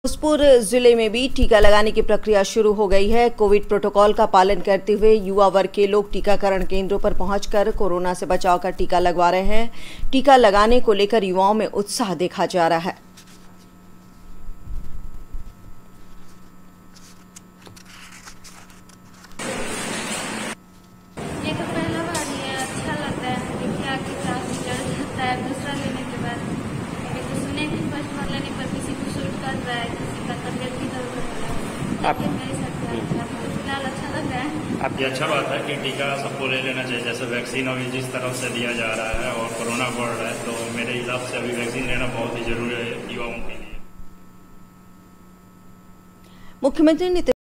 भोजपुर जिले में भी टीका लगाने की प्रक्रिया शुरू हो गयी है।
भोजपुर जिले में भी टीका लगाने की प्रक्रिया शुरू हो गयी है। कोविड प्रोटोकॉल को पालन करते हुए युवा वर्ग के लोग टीकाकरण केन्द्रों पर पहुंचकर कोरोना से बचाव का टीका लगवा रहे हैं। टीका लगाने को लेकर युवाओं में उत्साह देखा जा रहा है।